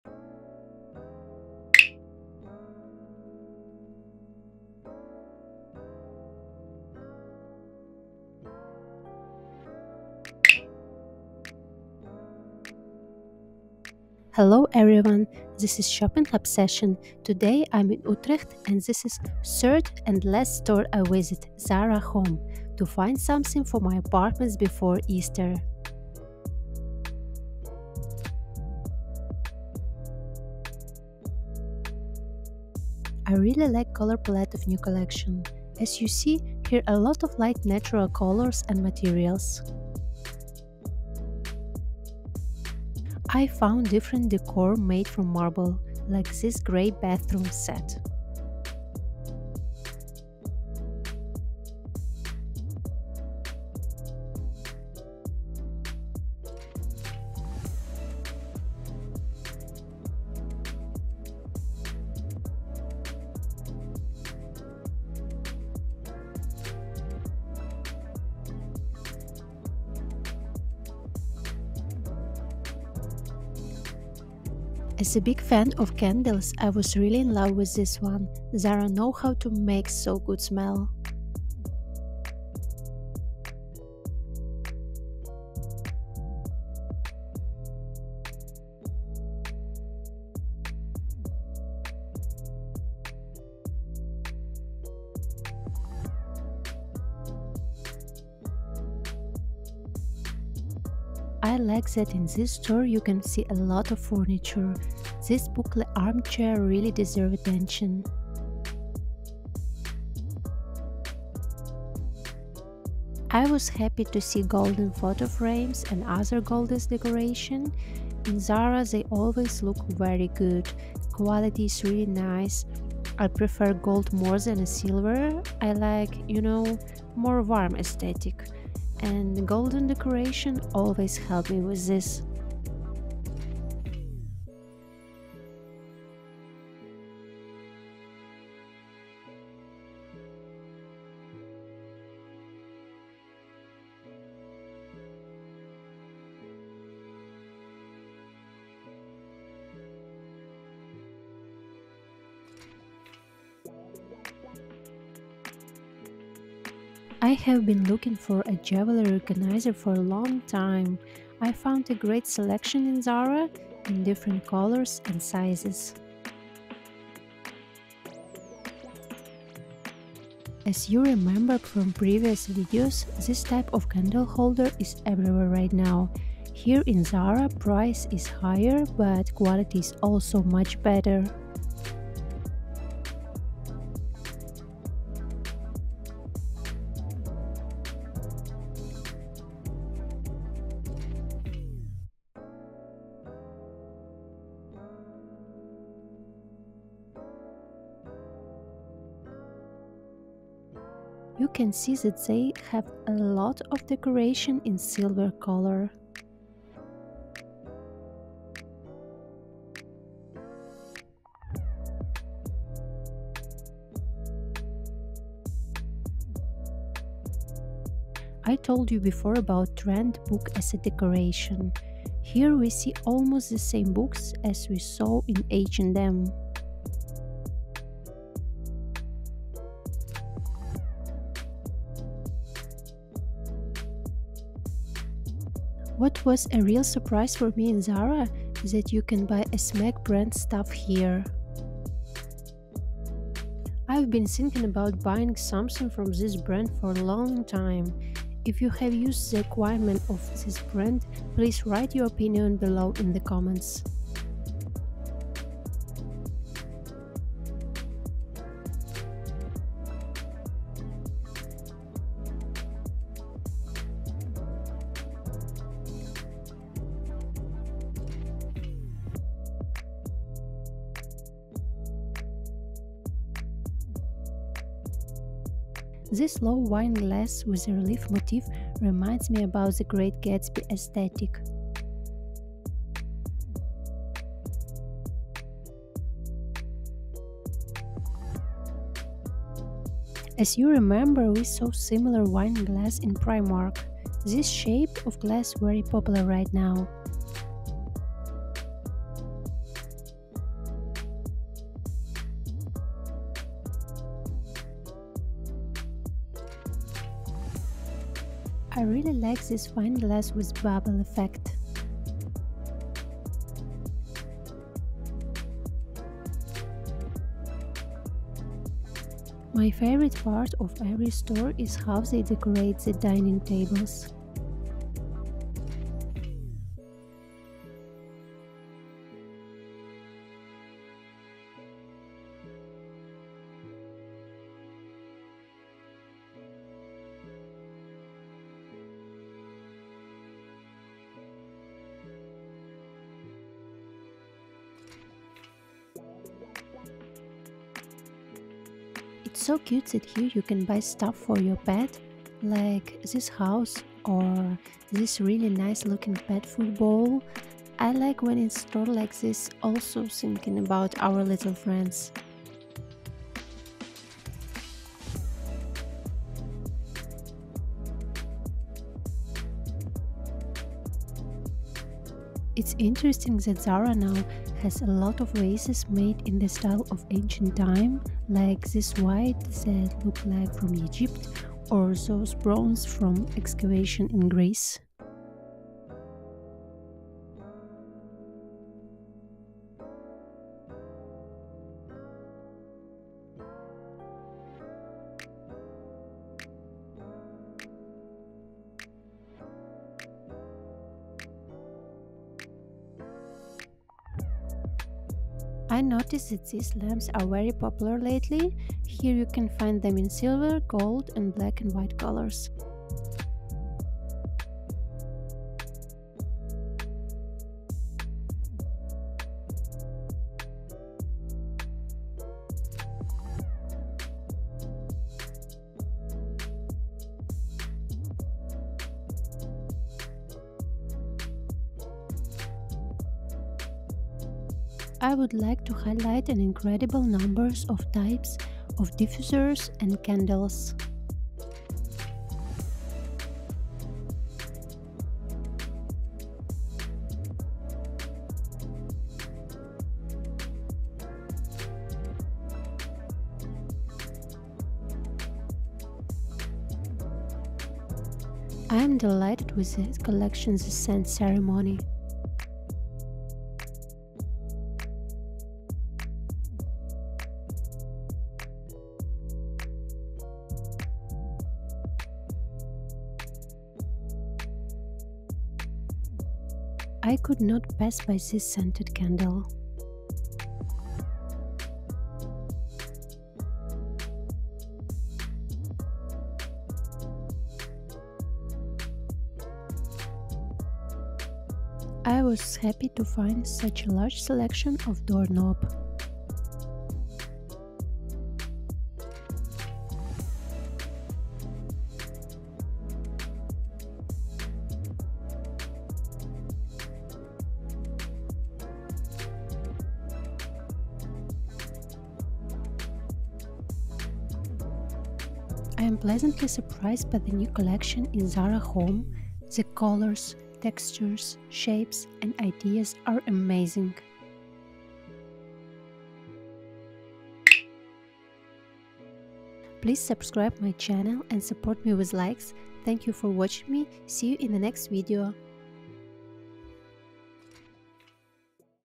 ZARA HOME SHOP WITH ME 🩷 WHAT'S IN STORE? EASTER DECOR COLLECTION MARCH 2024 UTRECHT SHOPPING VLOG
ZARA HOME March 2024
HOME DECOR COLLECTION
The latest trends in decor. Interior design.

UTRECHT
HOOG CATHARIJNE SHOPPING MALL